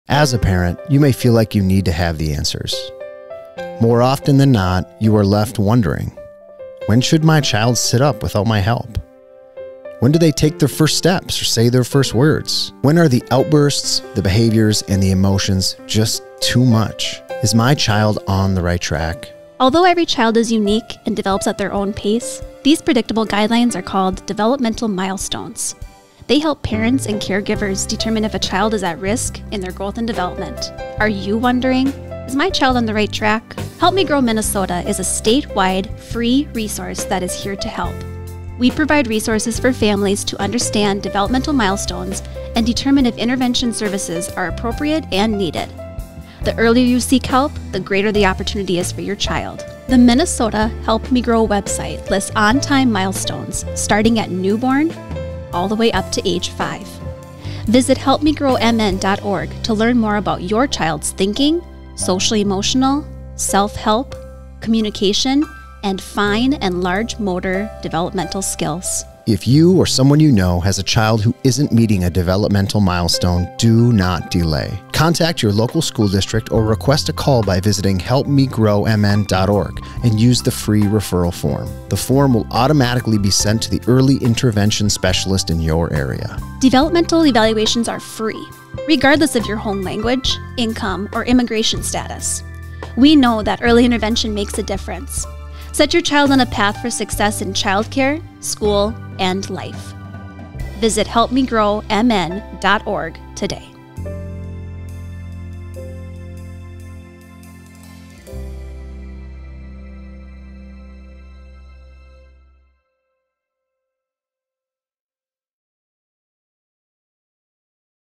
Refer a Child – Minnesota Help Me Grow, Region 6 IEIC
A peek into Early Intervention by Southwest West Central Intra-Agency Early Childhood Intervention Committee (IEIC) Region 6. Learn more about early childhood developmental milestones If you have concerns about a child from birth to age five years and think they might need extra help to learn and grow, don’t hesitate! The family will be contacted by the local school district to arrange for a screening or evaluation to determine if their child is eligible for Infant and Toddler Early Intervention or Preschool Special Education services in Minnesota.